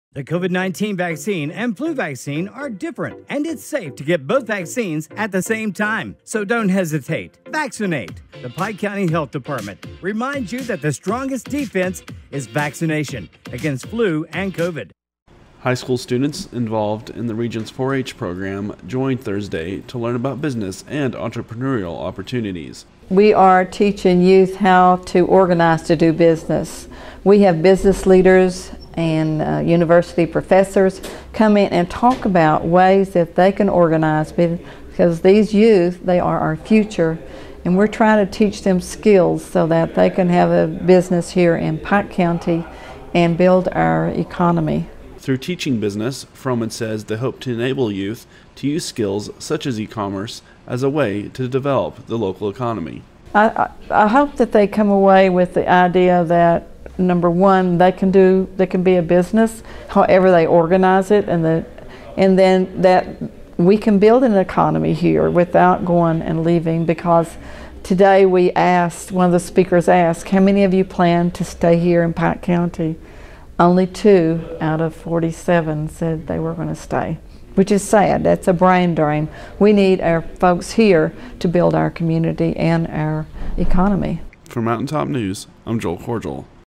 4-H Teaches Business to Local Schools
High school students involved in the region's 4-H program joined Thursday to learn about business and entrepreneurial opportunities.